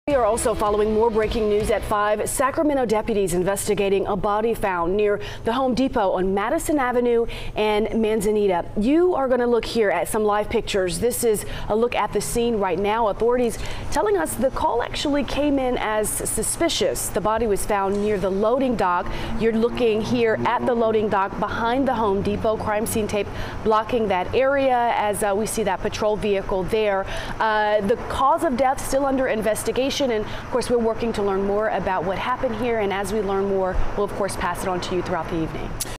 Deputies investigate body found near Carmichael Home Depot
The Sacramento County Sheriff’s Office is investigating after a body was found near a Home Depot in Carmichael.  The Home Depot is located on Madison and Manzanita avenues.